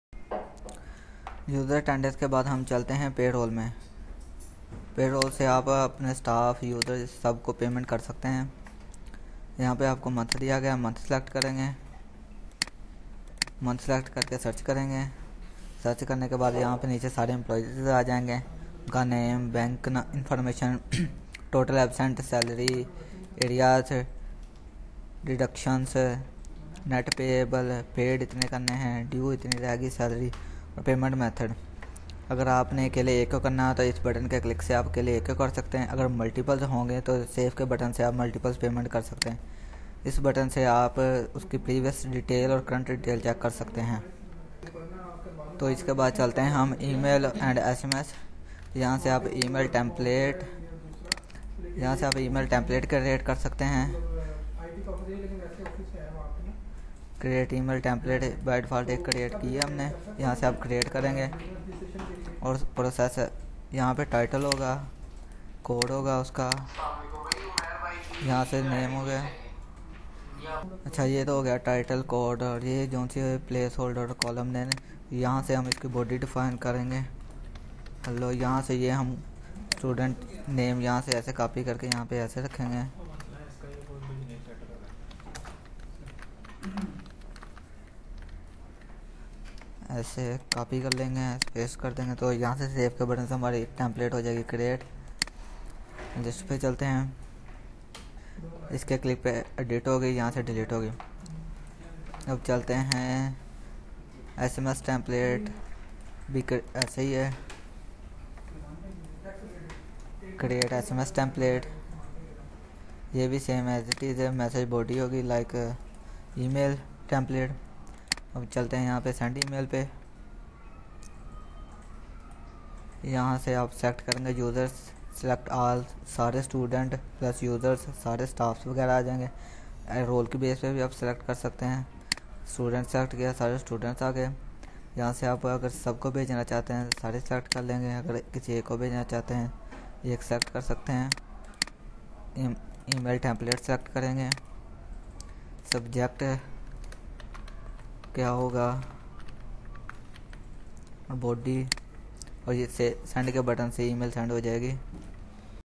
How to setup payroll, email, sms, template and send email | SumSchool
In this video you will learn how to setup payroll, email, sms, template and send email in SumSchool (School Management System)

Password: 123456